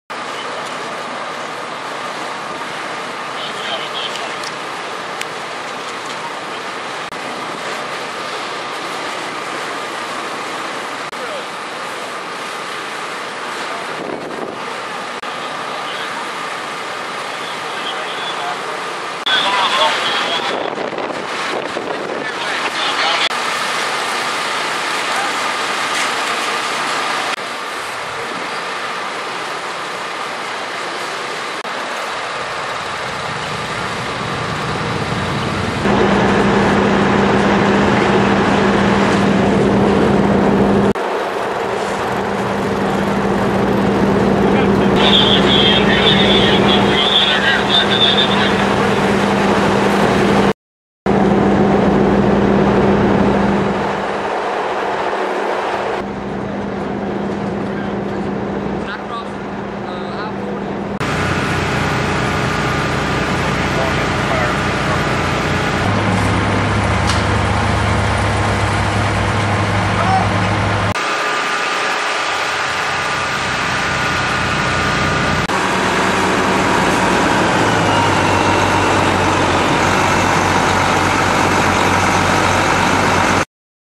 4-3-2016 Benson - Structure Fire
Outbuildings, a boat and a vehicle on Morgan Road, Benson shortly after midnight 4-3-2016.

Benson FD, Blackman's Crossroads FD, and Meadow FD along with Johnston County EMS and the Johnston County Fire Marshal's Office.